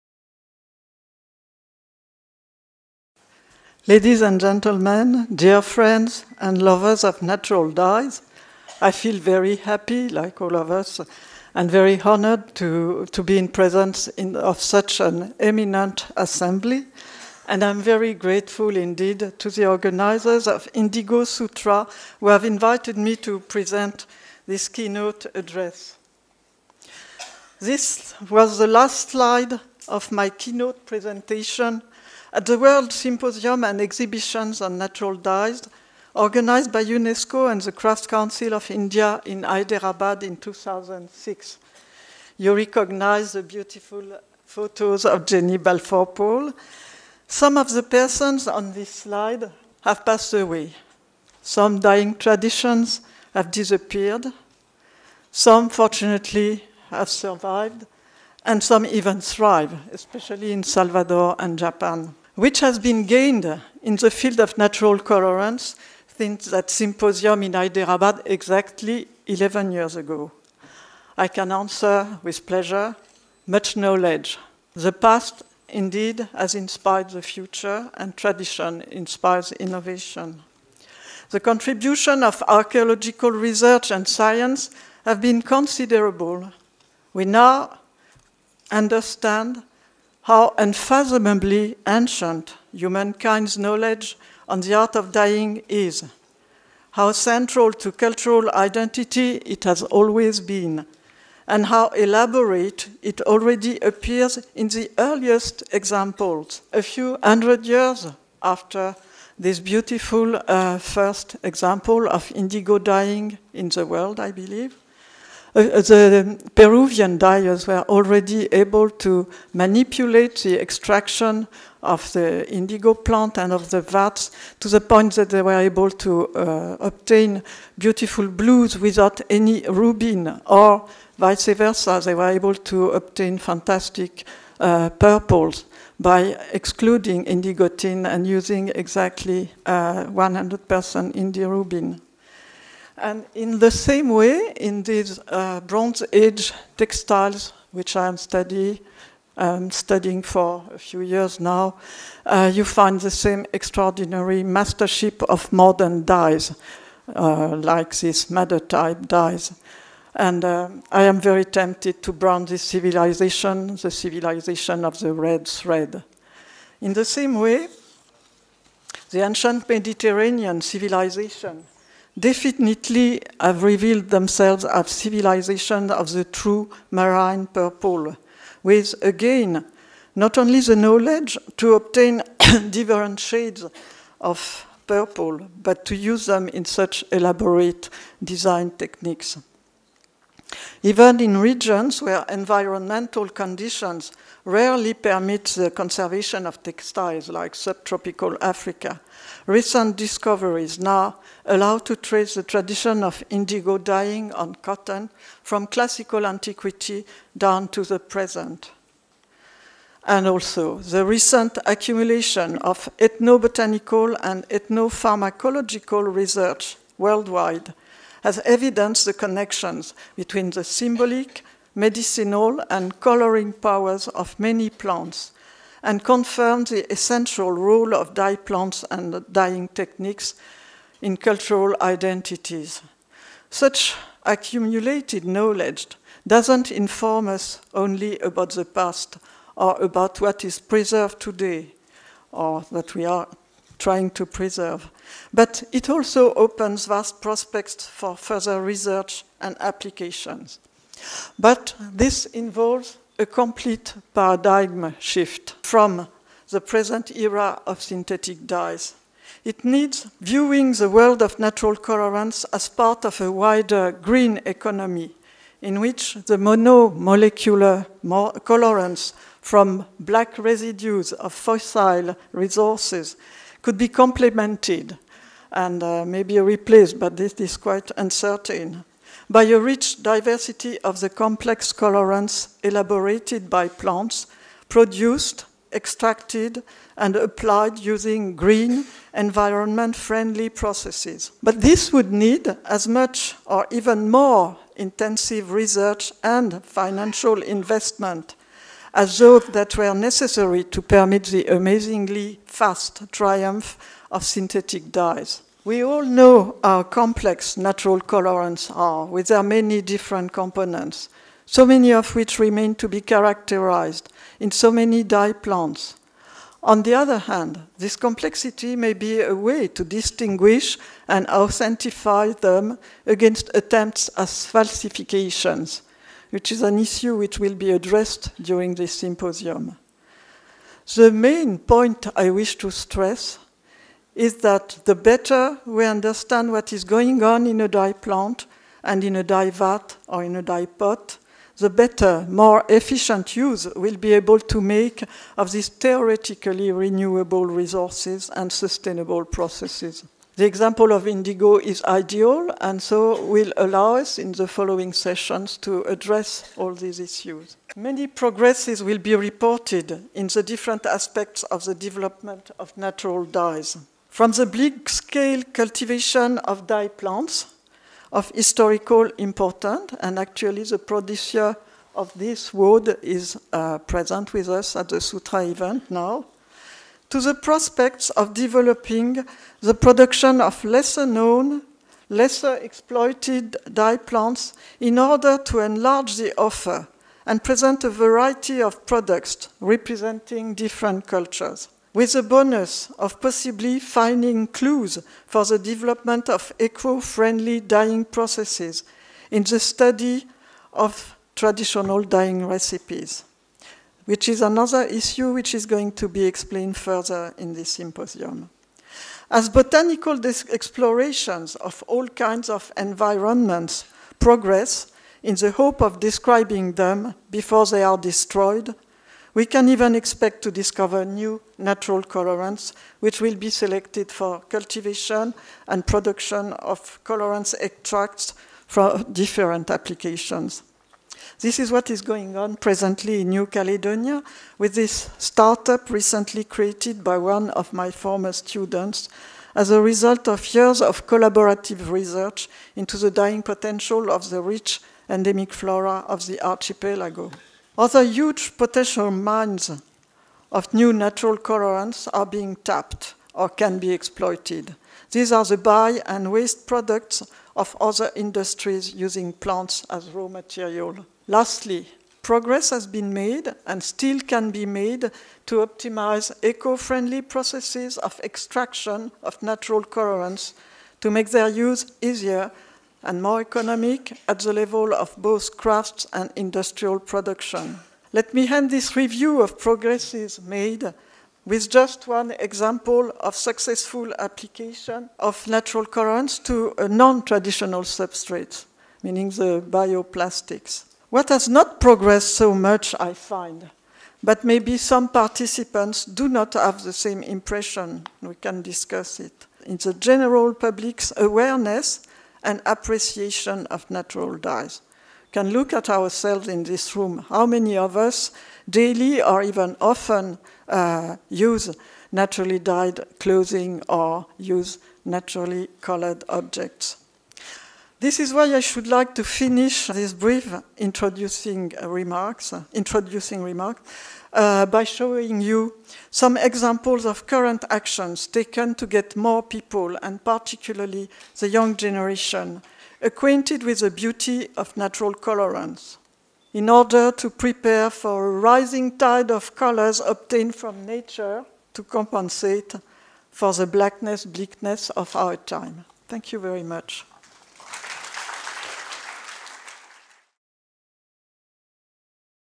Dr Dominique Cardon | Author and Researcher
Speaker : Dr Dominique Cardon, author of Natural Dyes : Sources, Tradition, Technology and Science

INDIGO SUTRA was an international event on the revival and resurgence of natural indigo.

The event took place in Kolkata, India from 9-18 November 2017 to celebrate Indigo, the worlds oldest, most widely used and unique dyestuff and the only natural blue dye which takes its very name from 'India'.

Comprising a seminar, workshops, demonstrations, exhibitions, documentary films, bazaar and textile tours, it attracted people from all over India and from every continent in the world.

The idea was to create awareness of reviving the cultivation of indigo in Bengal and other places where it had stopped due to political unrest or other reasons and also to encourage the use of natural indigo dyeing using natural methods.